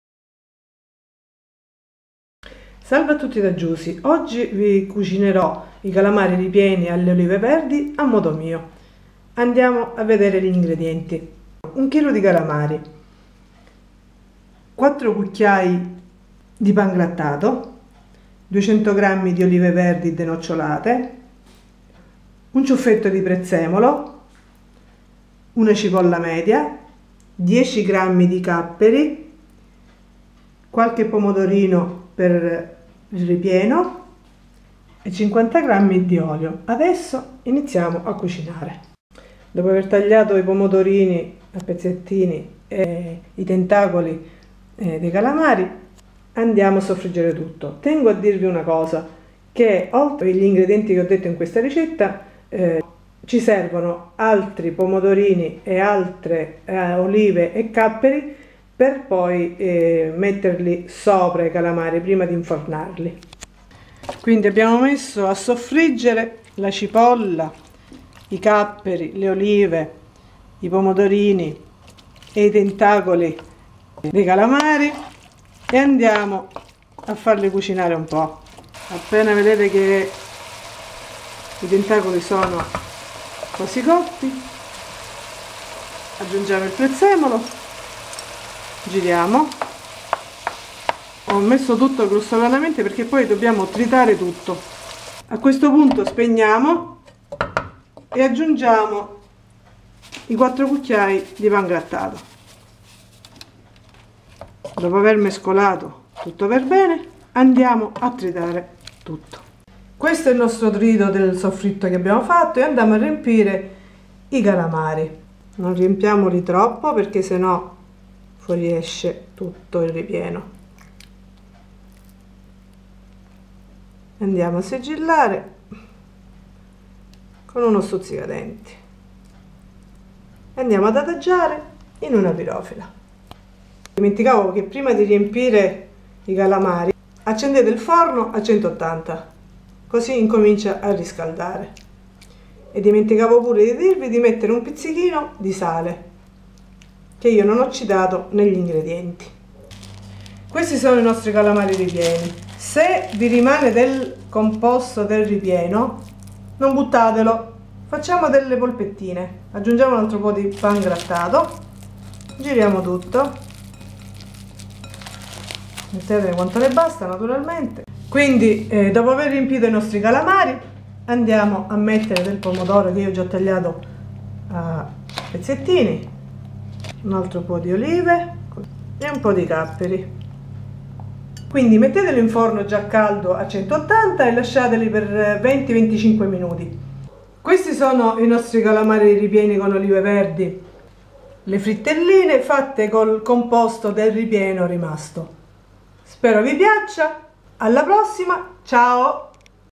Calamari ripieni con Olive Verdi
Ingredienti:
-1 Kg Calamari
-4 Cucchiai di Pan grattato
-200gr Olive Verdi +  50gr
-1 Ciuffetto di Prezzemolo
-1 Cipolla
-10gr di Capperi
-200gr Pomodorini + 50gr
-50gr di Olio EVO
-1 pizzico di sale

Spero che il video vi sia stato utile. Ciaooo !